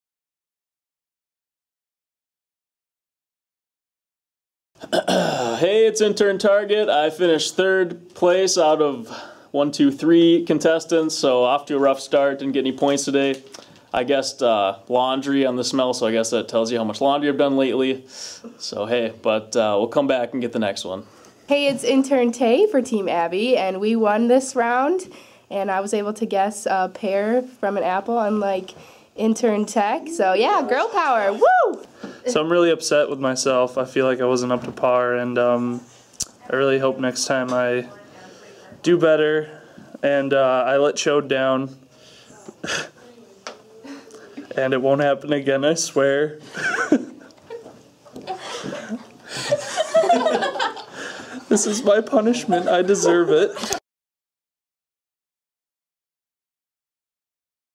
Intern Games 1: Intern Confessions
Hear from our interns on what they thought of the first challenge.